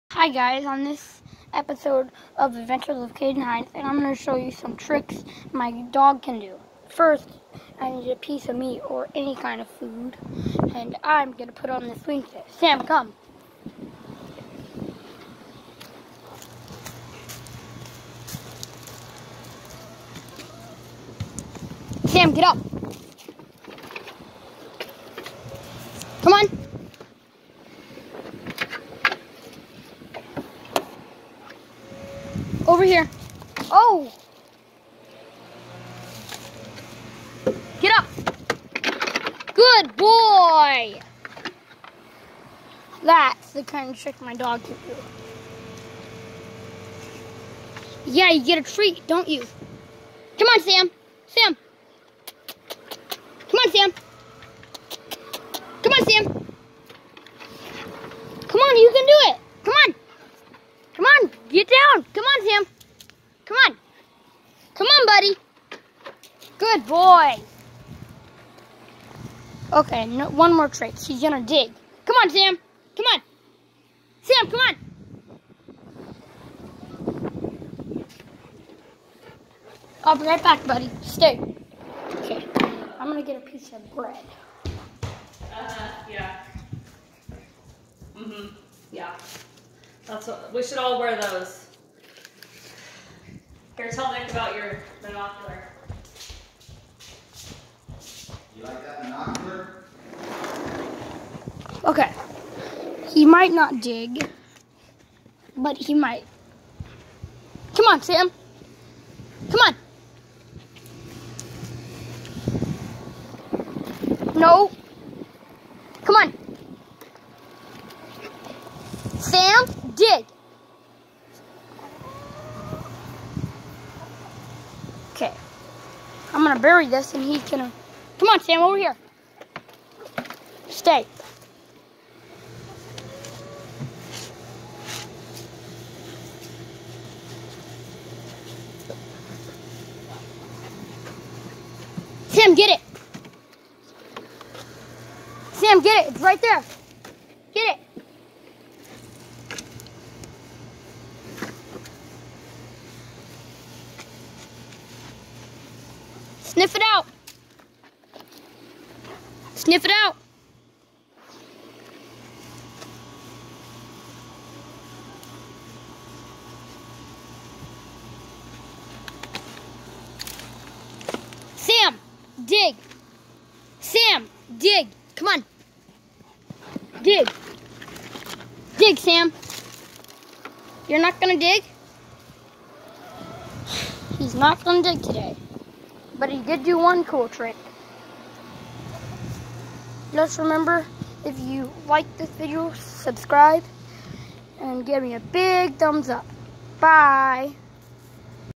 Sam the protector dog that does tricks.
About my Bernese mountain dog half lab that does tricks.